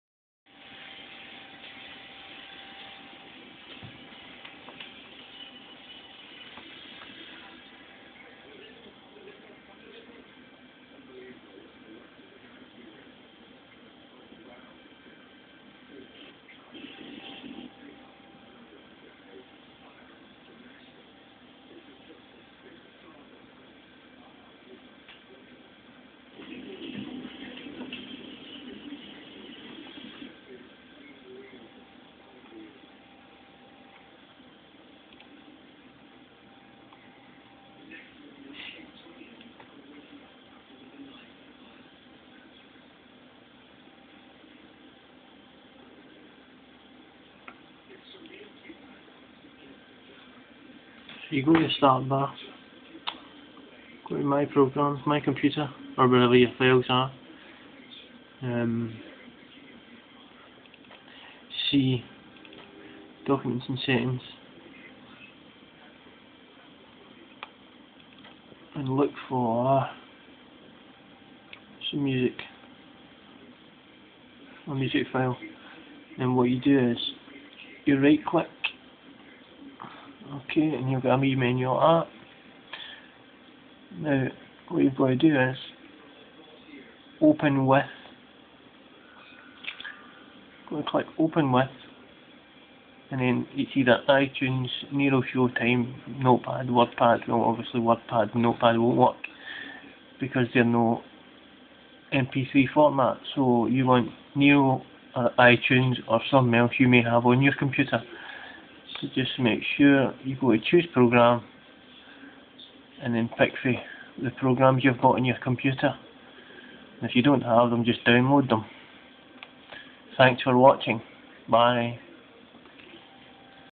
qikvideo-2010-04-15-21-16-50.3gp
how to open files on your pc with what program you want to open it with and not what the pc wants you to use.